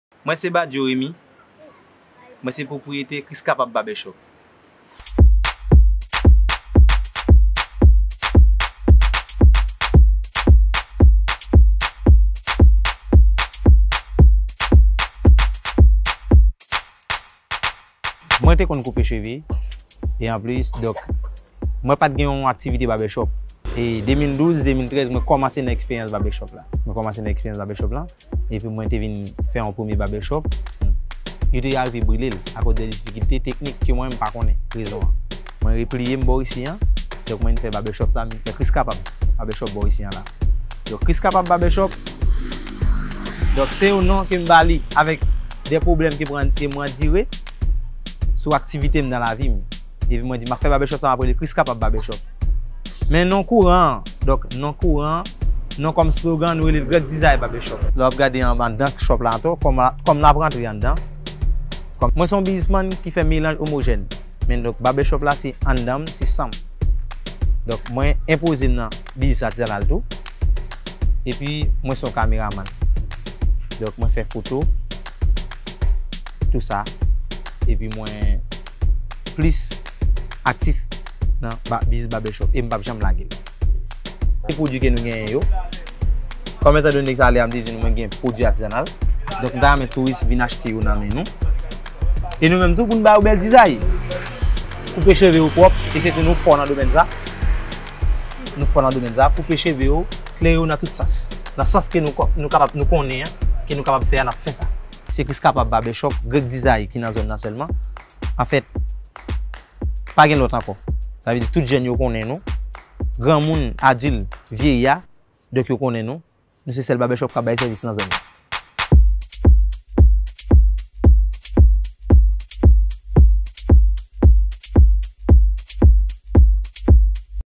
Meet the Locals – Episode 7: Rémy Badio, barber in Cayes-Jacmel
🤔 "What's it like in the life of a local?" If you've ever wondered, we have the answers! Our series, Meet the Locals, takes you through the lives of people all over Haiti and gives you a more intimate look at life on the island. Meet Rémy Badio, an artist and entrepreneur in Cayes-Jacmel. Rémy's creativity has no limits, and his barbershop is not only the best place to go when you need a new haircut. It's also the only place in this area where you can find such a creative and genuine spirit.